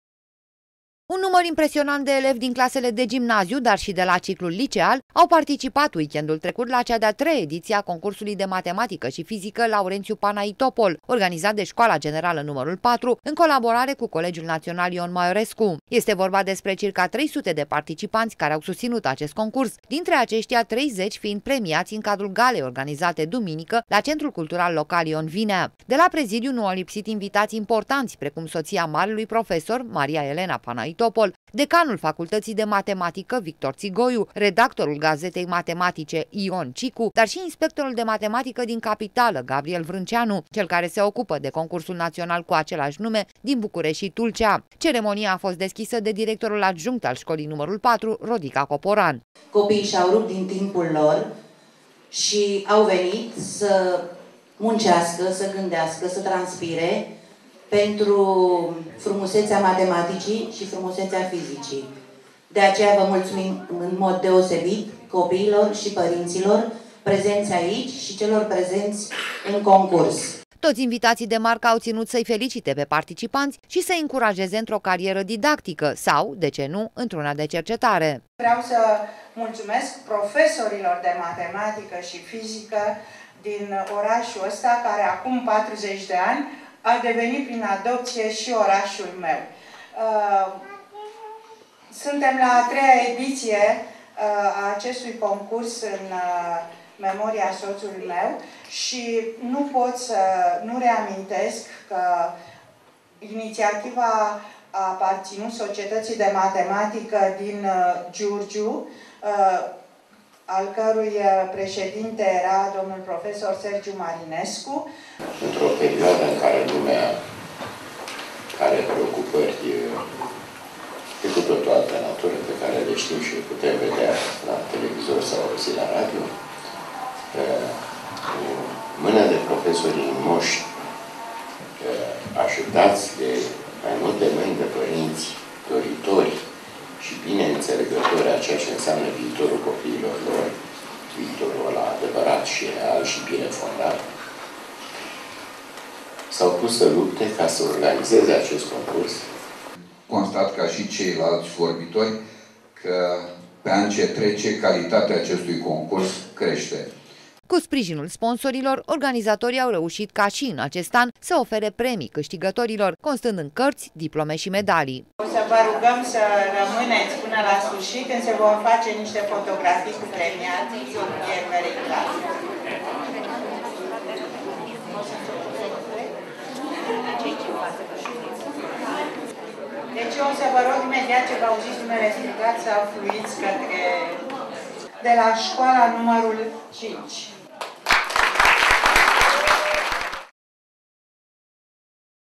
Concurs de nivel înalt
A devenit deja un concurs de notorietate pentru sistemul de invatamant din judet, dar si in plan national acolo unde se vorbeste la mod laudativ despre Giurgiu si elevii sai. Concursul judetean de matematica si fizica Laurentiu Panaitopol, desfasurat weekendul trecut la scoala nr 4, s-a bucurat de o largă participare, dar si de un nivel calitativ mai crescut, asa cum au apreciat organizatorii, in cadrul festivitatii de premiere a celor mai buni concurenti.